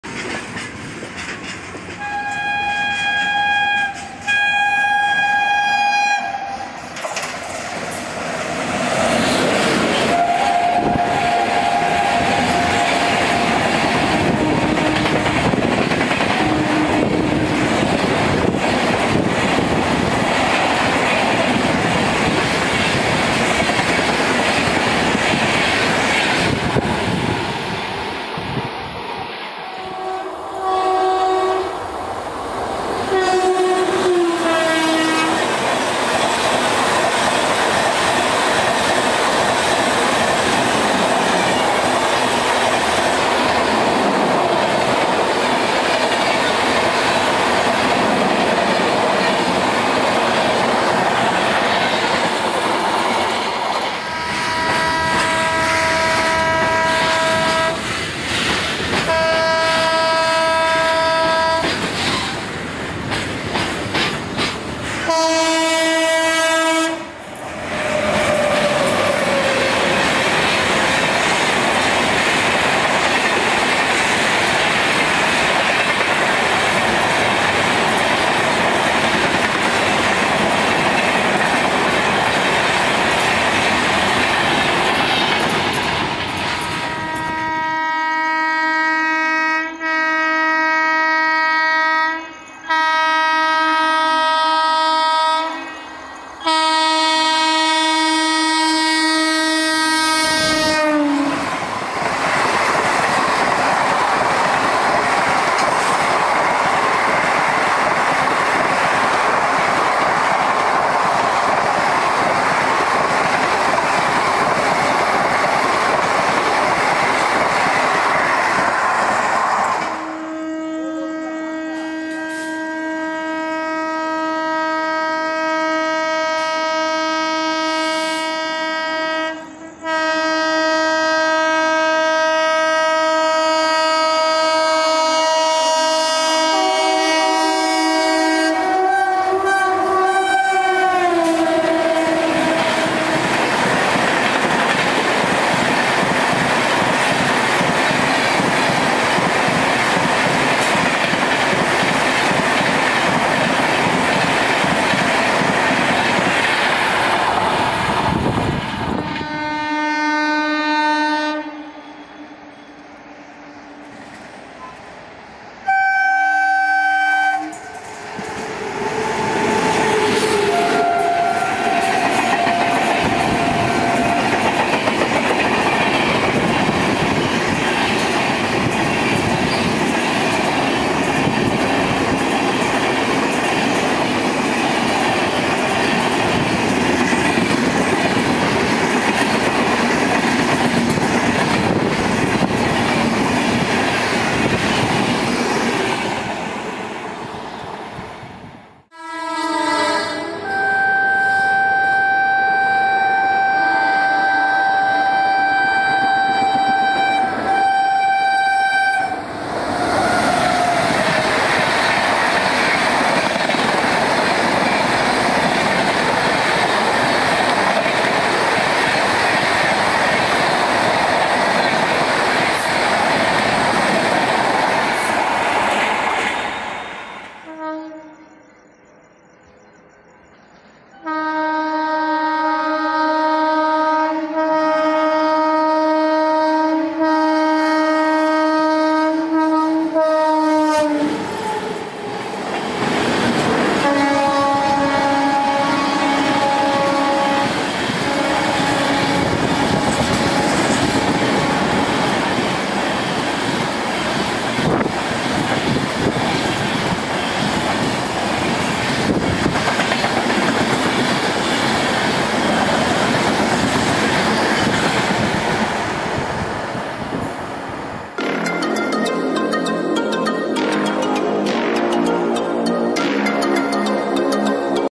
[8 in 1] HIGH SPEED Back to Back 130 Kmph Evening Trains in SER |Hamsafar + Ispat+Dhauli+Mysore SF.
HIGH SPEED Back to Back 130 Kmph Evening Trains in SER
1.22863/Howrah - Yesvantpur AC SF Express
2.02983/Bengaluru Cant. - Agartala SF Special
3.20890/Tirupati - Howrah Humsafar Express
4.22888/Yesvantpur - Howrah SF Express
5.12822/Dhauli Express
6.22817/Howrah - Mysuru Weekly SF Express
7.12872/Ispat Express
8.Howrah - Kanniyakumari Superfast Express